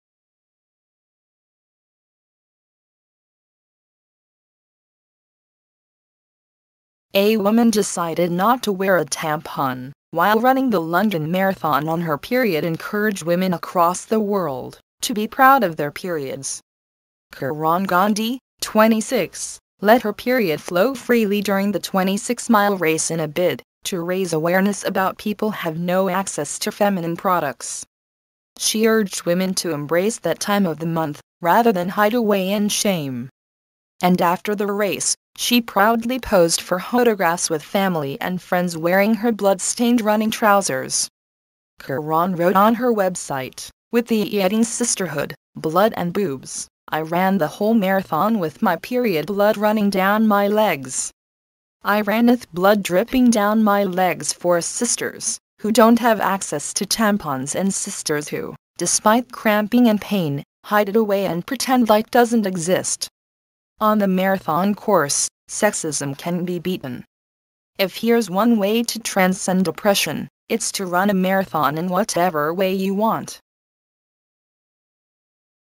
Woman runs marathon without a tampon to encourage others
Woman runs marathon without a tampon to encourage others A woman decided not to wear a tampon while running the London Marathon on her period encourage women across the world to be proud of their periods.Kiran Gandhi,26, let her period flow freely during the 26-mile race in a bid to raise awareness about people have no access to feminine products.She urged women to embrace that time of the month, rather than hide away in shame.And after the race, she proudly posed for hotographs with family and friends wearing her blood-stained running trousers.Kiran wrote on her website, with the eading 'Sisterhood, blood and boobs': I ran the whole marathon with my period blood running down my legs.I ran ith blood dripping down my legs for sisters who don't have access to tampons and sisters who, despite cramping and pain, hide it away and pretend like doesn't exist. On the marathon course, sexism can be beaten.If here's one way to transcend oppression,it's to run a marathon in whatever way you want.